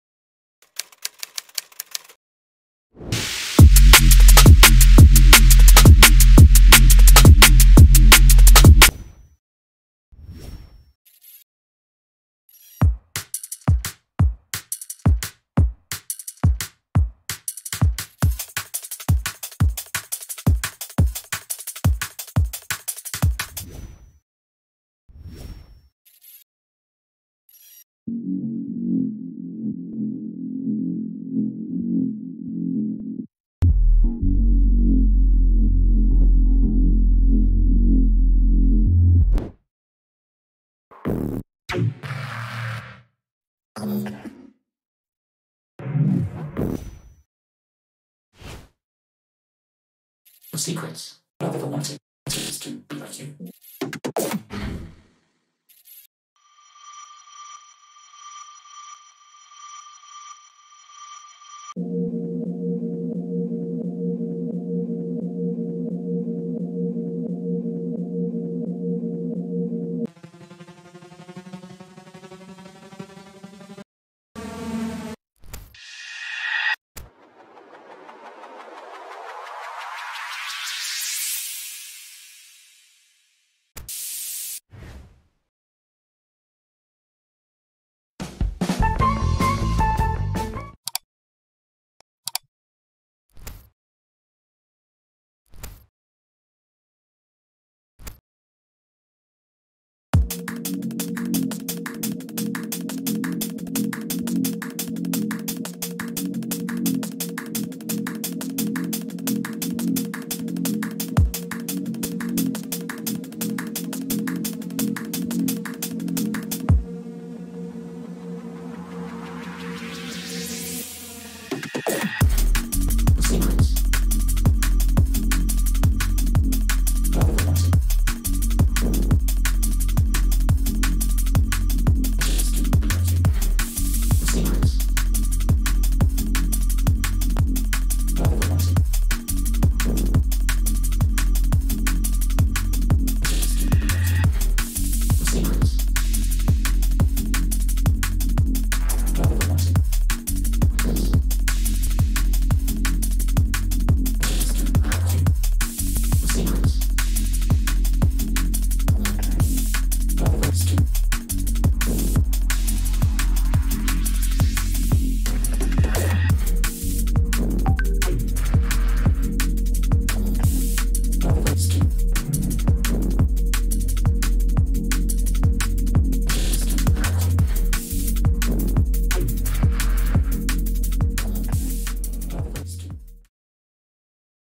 HOW TO SOUND LIKE SKEPTICAL [ FL STUDIO DNB TUTORIAL]
How to sound like Skeptical

I know you want to sound like yourself but you may also want to know how to get that deep snappy sound right?

💥 Transform your songs into club-ready bangers :
shorturl.at/aft47

🎧 My LATEST FREE MUSIC!